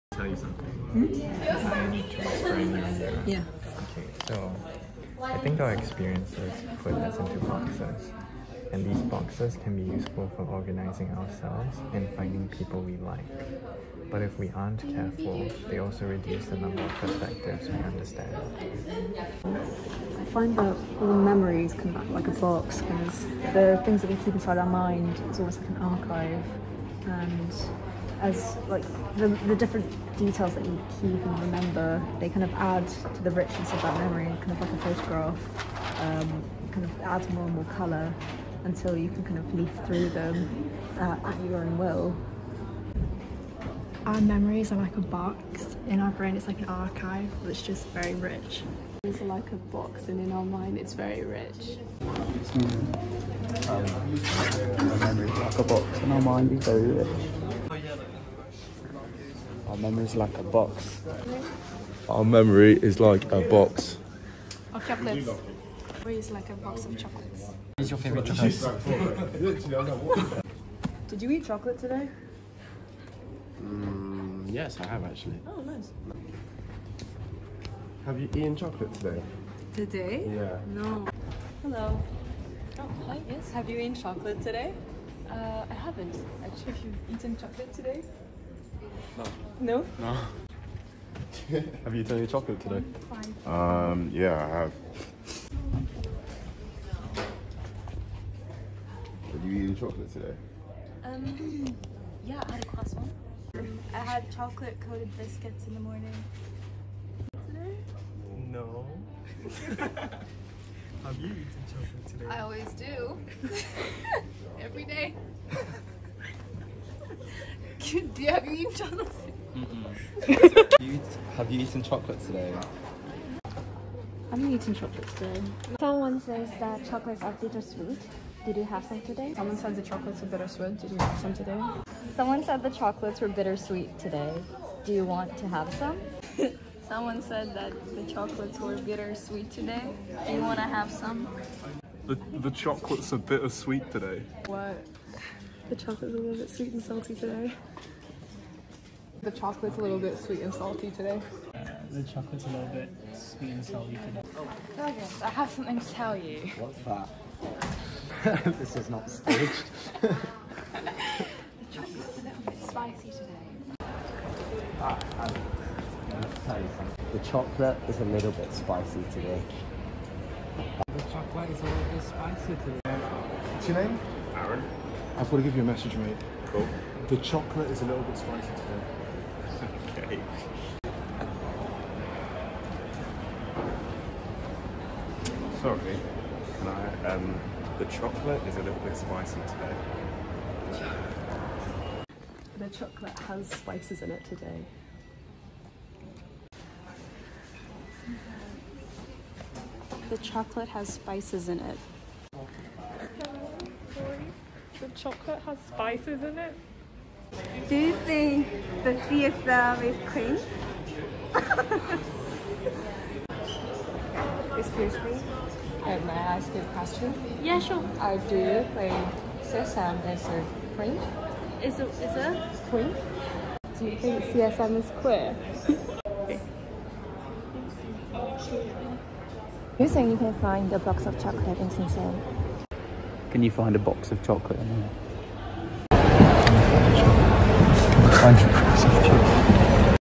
Whisper Box
I&S_ week 3 outcome

Using the game "Chinese Whisper" to explore how message become distorted